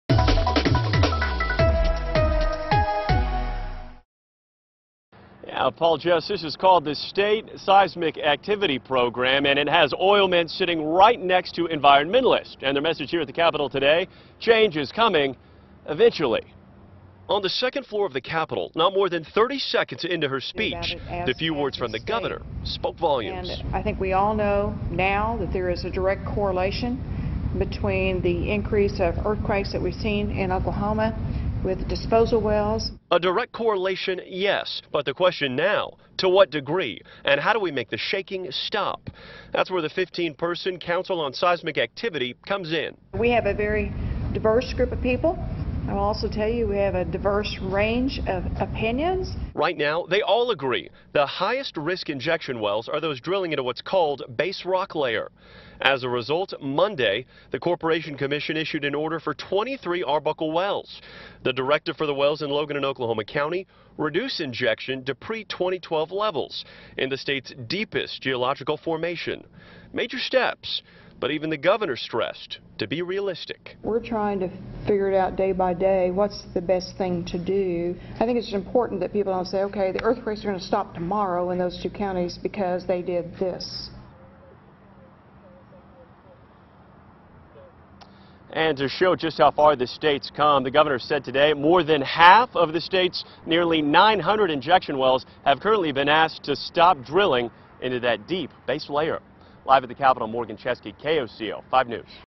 Fallin, oilmen, environmentalists meet to try to make the shaking stop
Gov. Mary Fallin met with her team of oilmen and environmentalists to try to figure out ways to protect Oklahomans and their property from the earthquakes that she says is directly correlated with disposal wells.